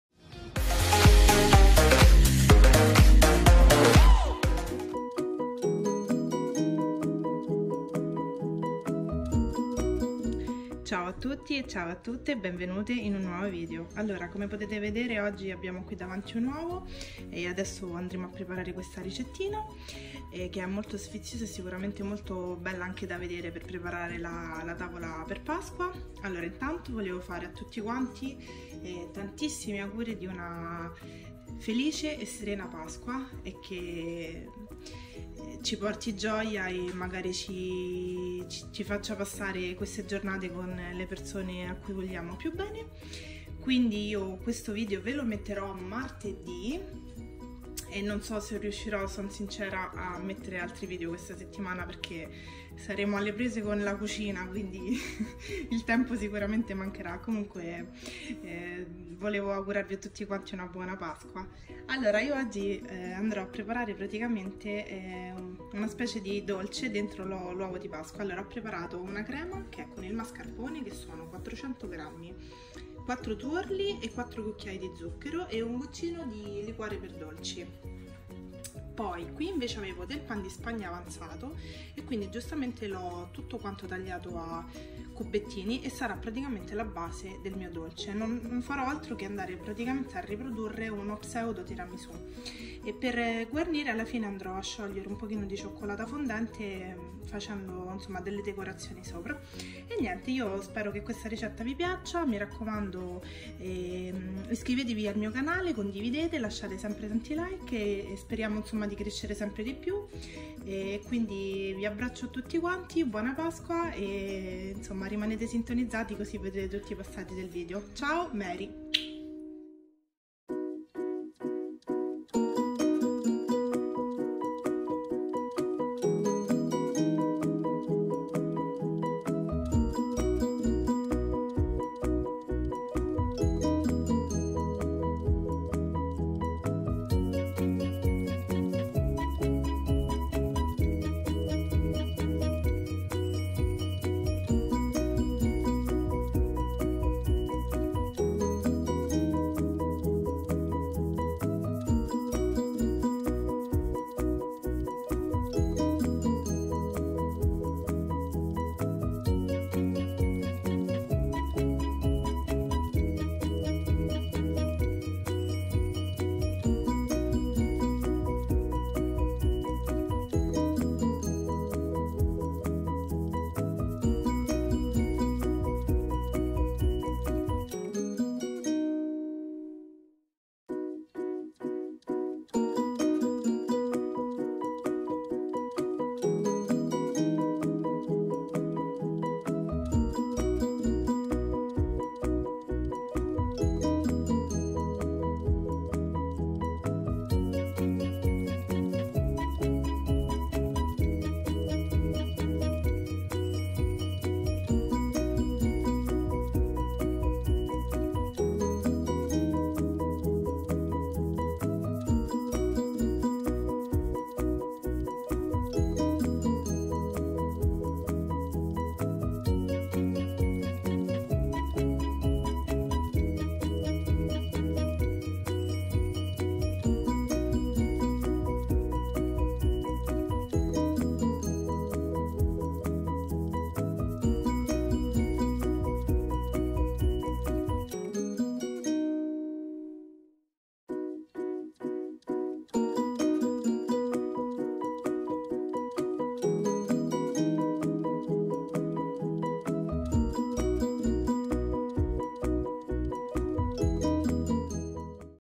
Uova di Pasqua Ripiene 🐣 con Pan di Spagna, Crema di Mascarpone e Liquore per Dolci
Oggi vi mostro la mia ricetta per preparare delle bellissime e deliziose Uova di Pasqua Ripiene. Se ti piacciono le mie ricette, sostienimi iscrivendoti al mio canale, lasciando like ai video e condividendo le ricette con i tuoi amici e le tue amiche :-) Ed ora allacciamo i grembiuli ed andiamo a preparare le Uova di Pasqua Ripiene! qui di seguito vi lascio gli ingredienti

📋 Ingredienti per la preparazione della crema:

- 4 Tuorli
- 400g di Mascarpone
- 4 Cucchiai di Zucchero semolato
- Liquore per dolci q.b.

📋 Ingredienti per la base:

- Pan di Spagna tagliato a cubetti

📋 Ingredienti per la decorazione:

- Cioccolato fondente sciolto a bagnomaria q.b.